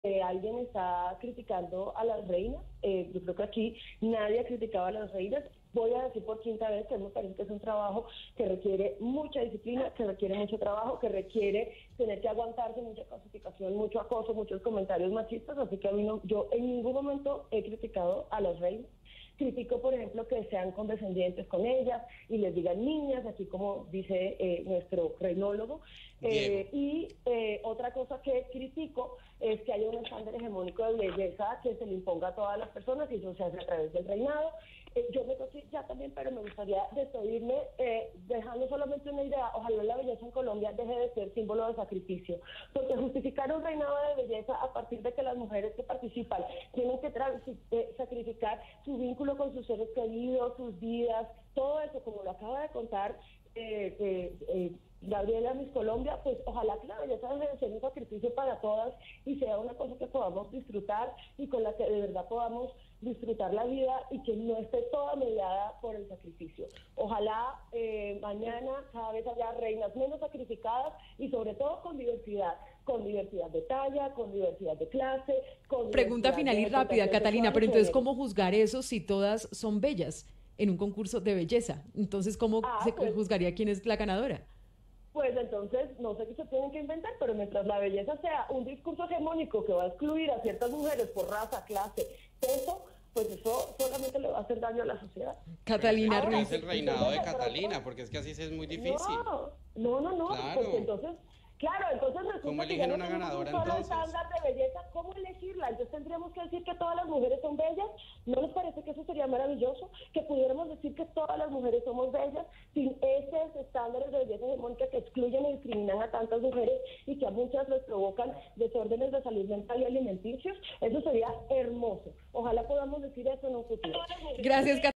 ¿No les parece hermoso poder decir que todas las mujeres somos bellas?: Catalina Ruiz
La feminista abogó por un nuevo modelo de certamen y dijo que  mientras el concurso sea excluyente solamente va a hacerle daño a la sociedad.
Entrevista emitida el 8 de noviembre de 2019